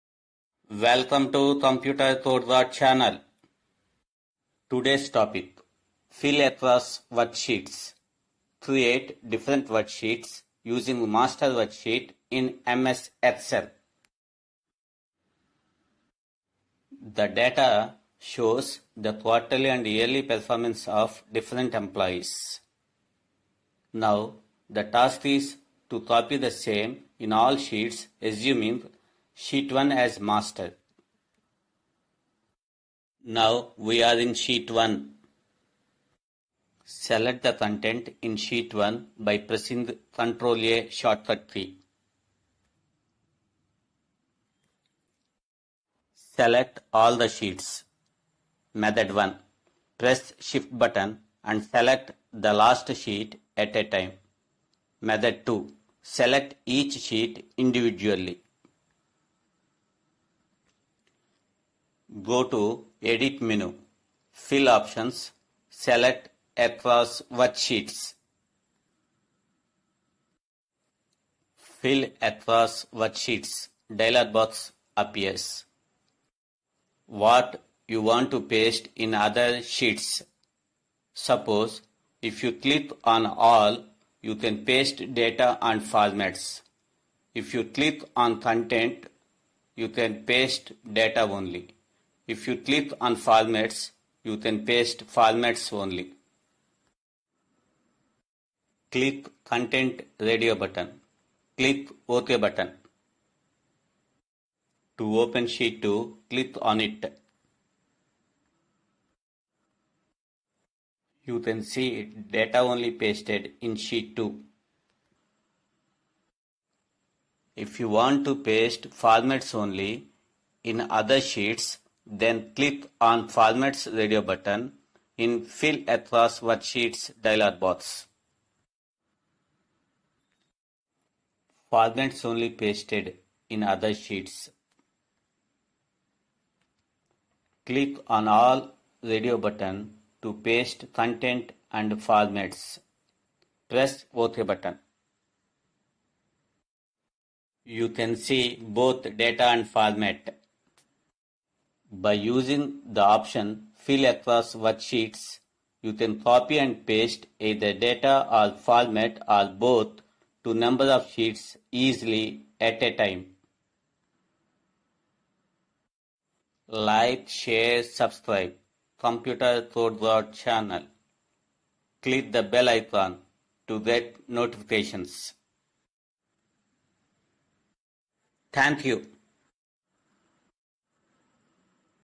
Fill Across Worksheets in MS Excel |Time Saving
fill data automatically  in multiple sheets in ms excel (fill across worksheets)
. time saving feature
.  good option for data entry work

Computer Code Guard
channel